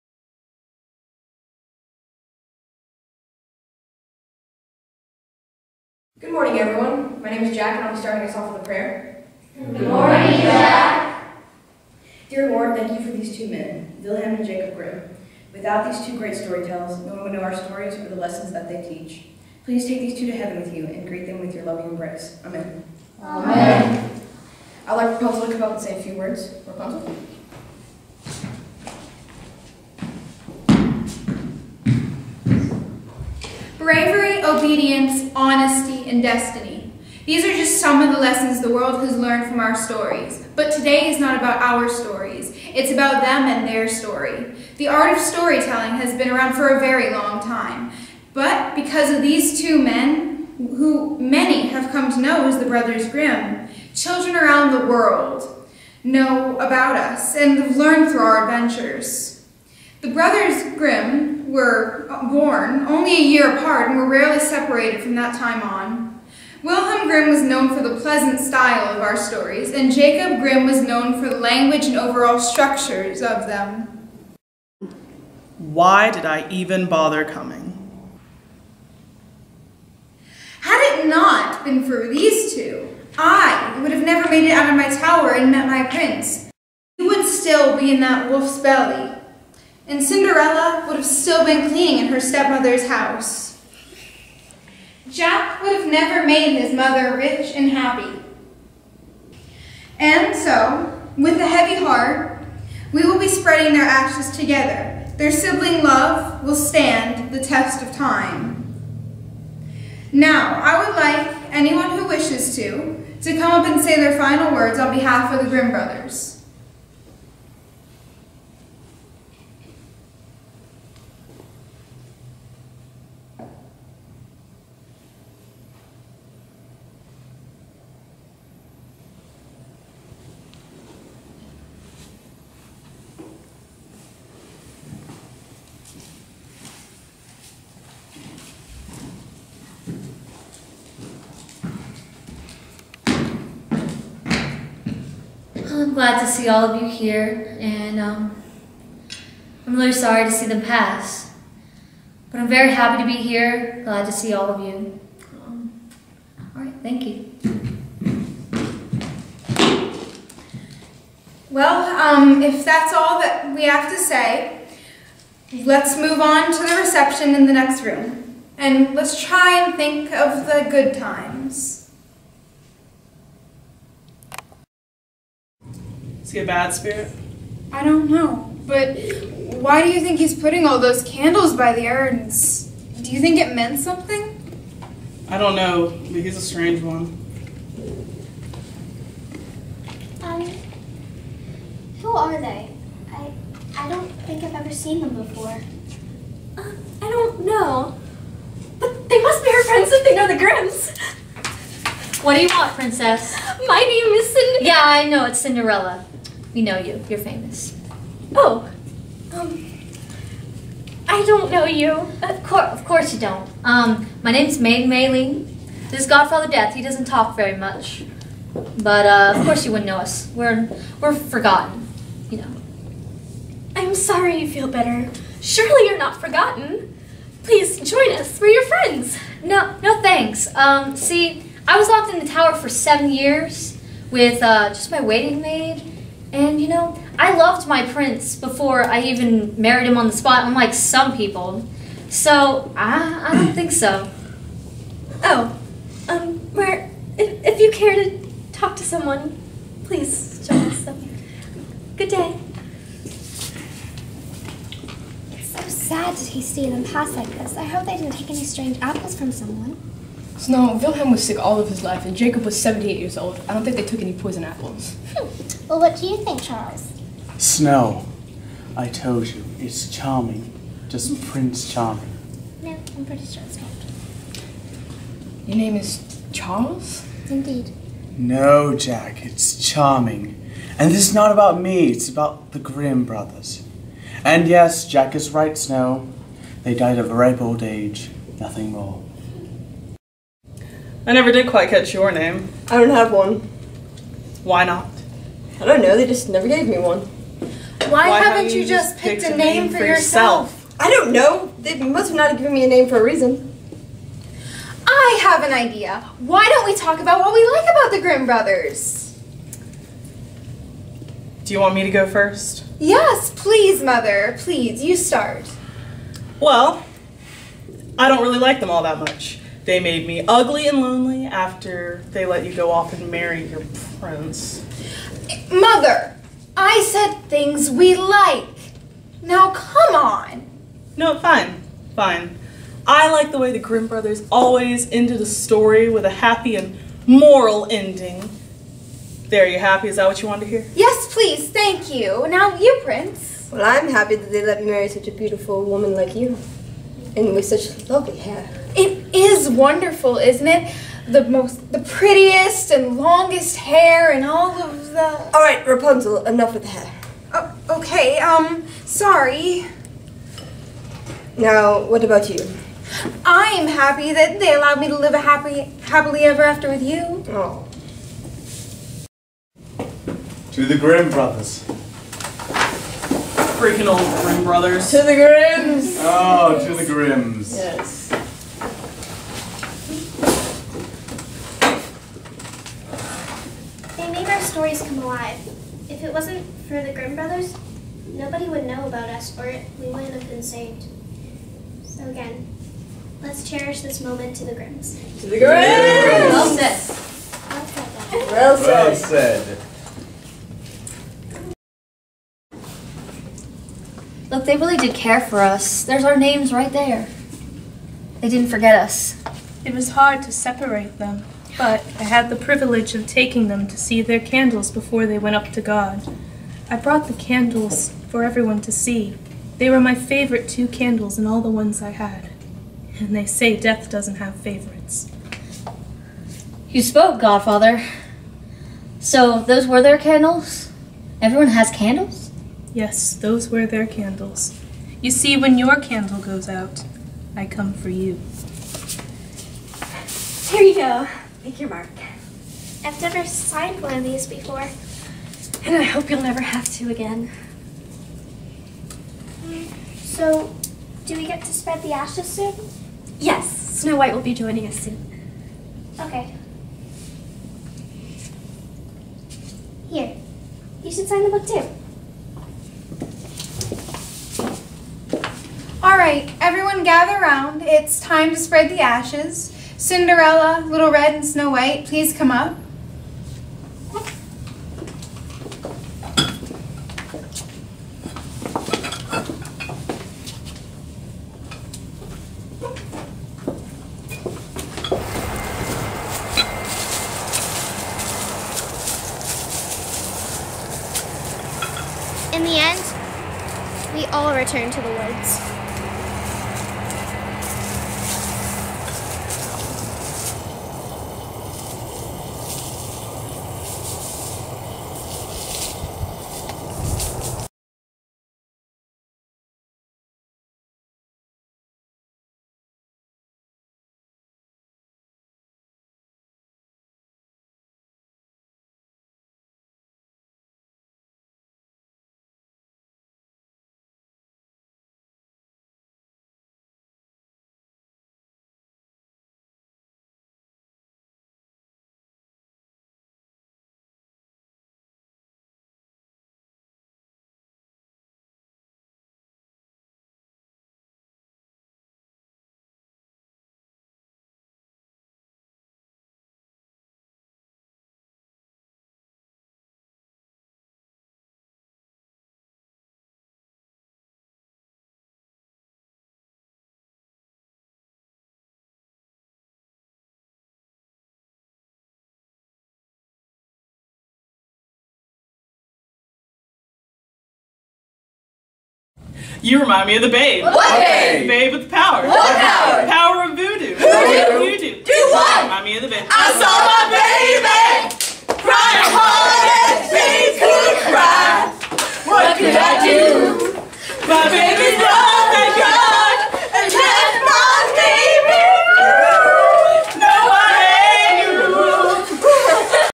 A Grimm Ending
This was my final English 101 project for USC Lancaster.

Thanks to all that acted, edited and filmed.Thanks to Riverview Presbyterian Church for the location.

Karly Ross, December 2014